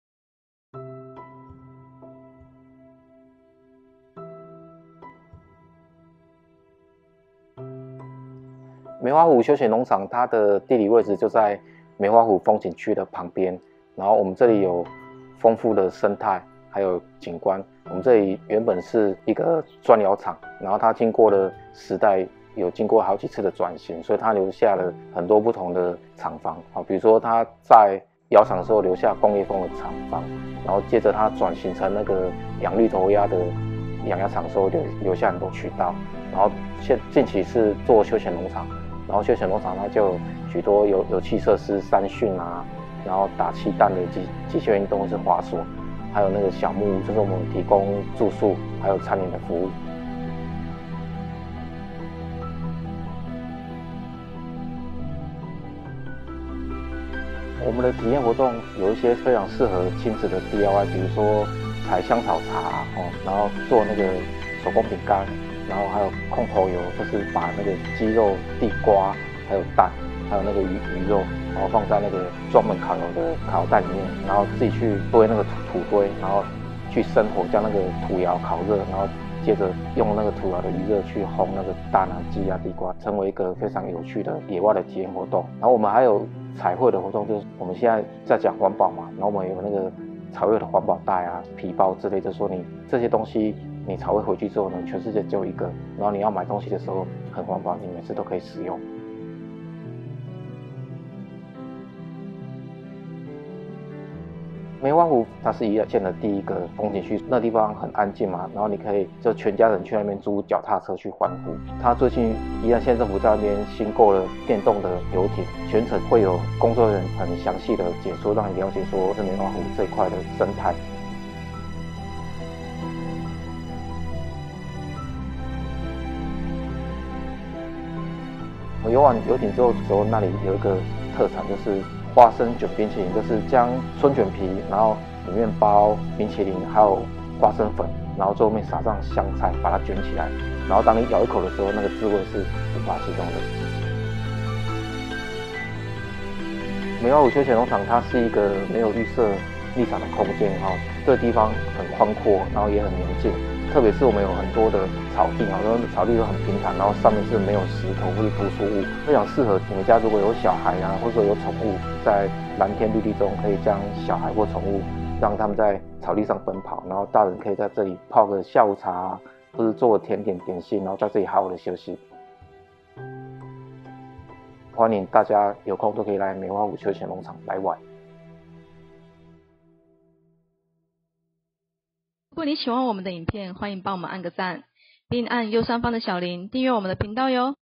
《壹圓》體驗古時的農村生活︱梅花湖休閒農場︱宜蘭系列民宿#41
梅花湖是宜蘭第一個風景區，而休閒農場內，在過去的歲月中磚窯廠亦曾輝煌過。

Meihua Lake is the first scenic spot in Yilan County. Among the leisure farms, the brick kiln factory has been glorious in the past few years.